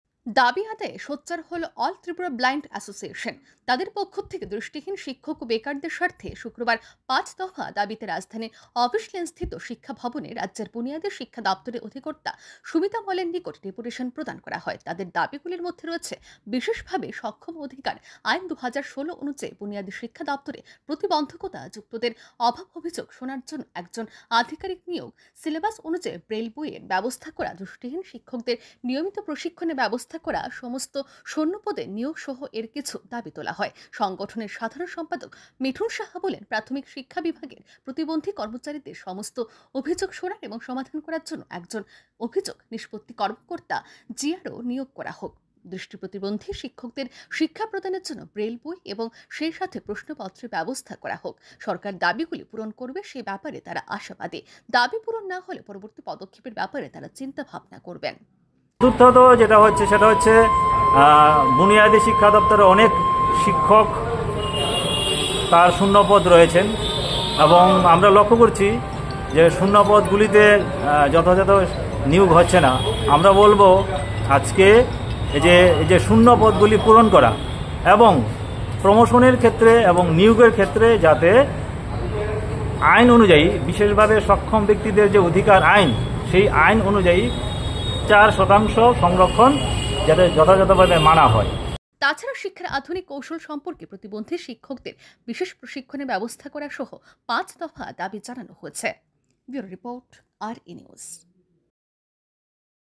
৫ দফা দাবিতে শিক্ষা ভবনে দৃষ্টিহীনদের ডেপুটেশন
৫ দফা দাবিতে শিক্ষা ভবনে দৃষ্টিহীনদের ডেপুটেশন